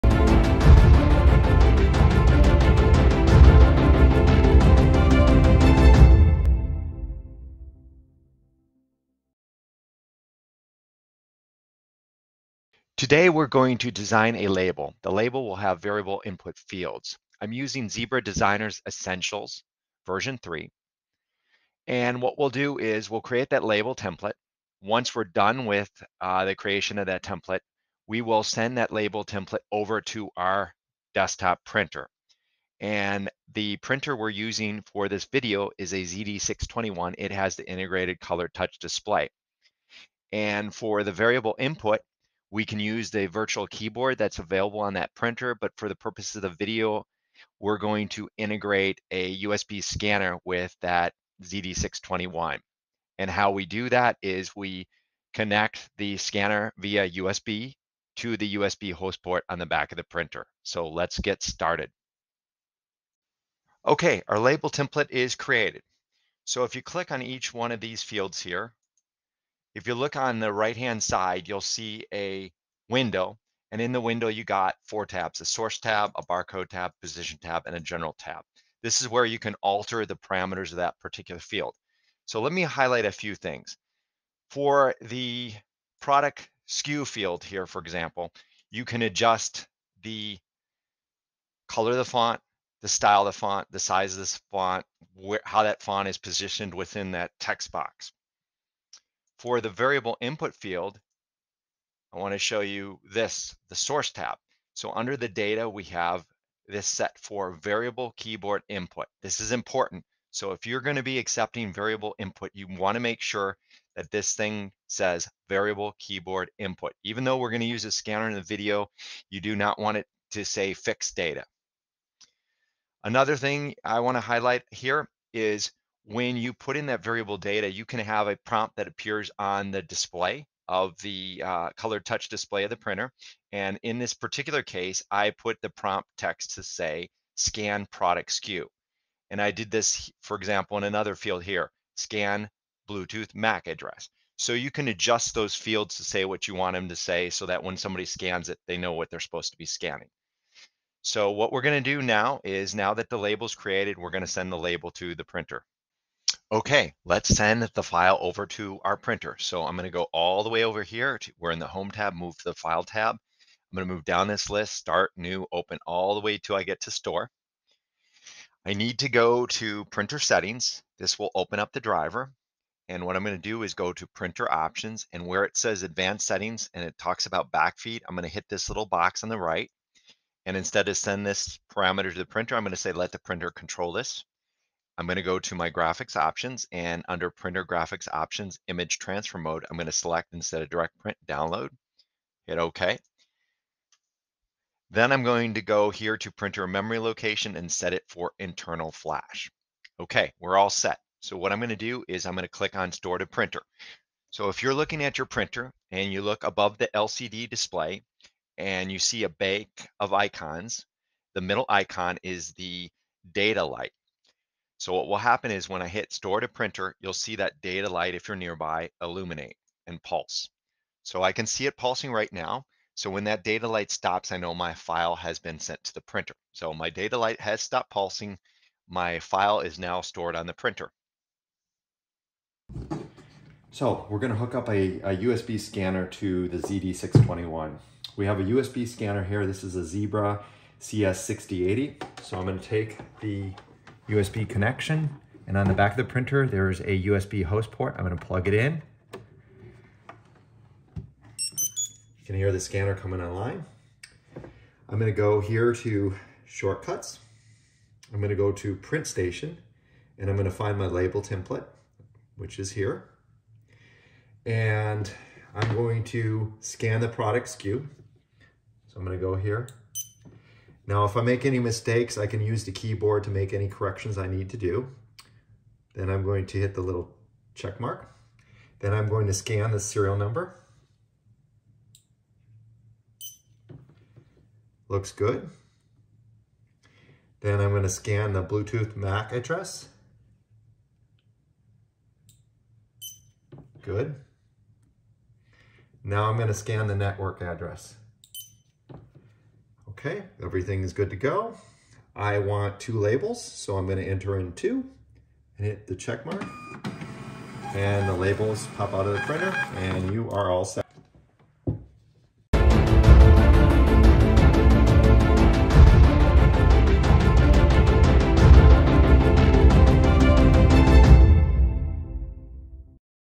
Variable Input Label: ZD621T & CS6080 Scanner | Zebra
How to create a label in ZebraDesigner Essentials that has variable input fields and how to use a Zebra USB scanner and Zebra desktop printer to add that input to the label.